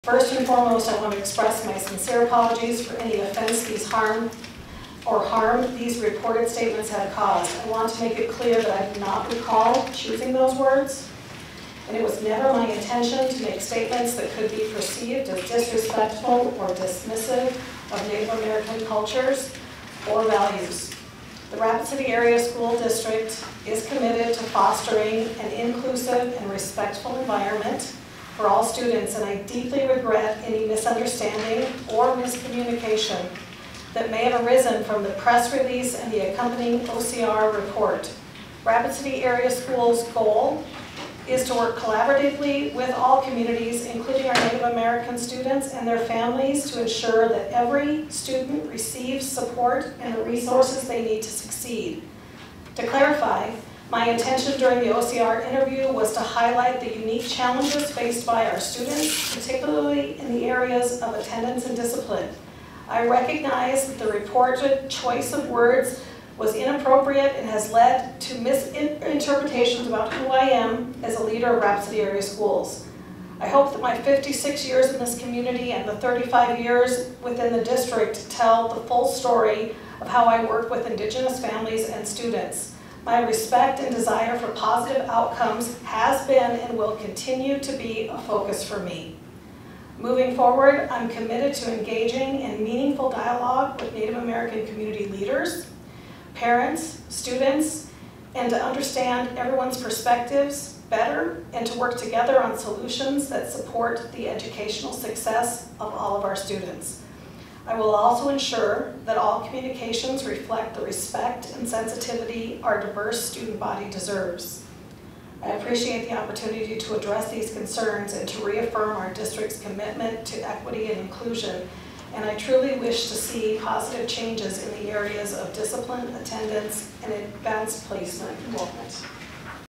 RCAS superintendent ORC comments apology
Apology by Nicole Swigart during the RCAS Board of Education meeting.